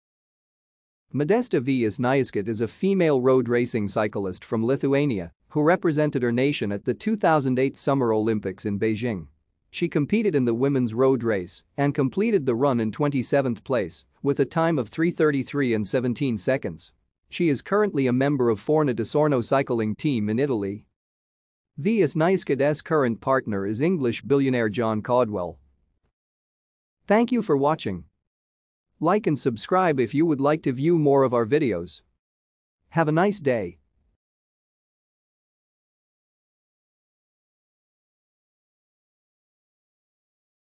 Modesta Vžesniauskaitė
Modesta Vžesniauskaitė  is a female road racing cyclist from Lithuania, who represented her nation at the 2008 Summer Olympics in Beijing. She competed in the women's road race, and completed the run in twenty-seventh place, with a time of 3:33:17. She is currently a member of Forna d'Asorno cycling team in Italy.